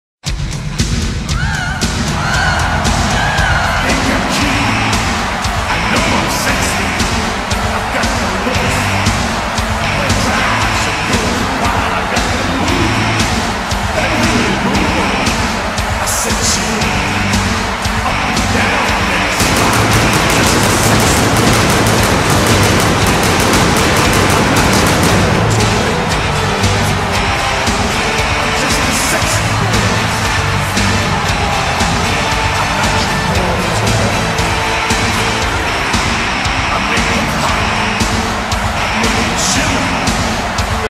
WWE Shawn Michaels Entrace Stage
Happy Birthday the one and Only Heart Break Kid, and one of the men that made me watch Wrestling, Shawn Michaels! Also he gave us the best match in WWE History (VS Undertaker Wrestlemania 25)
Thanks to: Pacomnr for The Stage, and the devilIncanate for the Graphics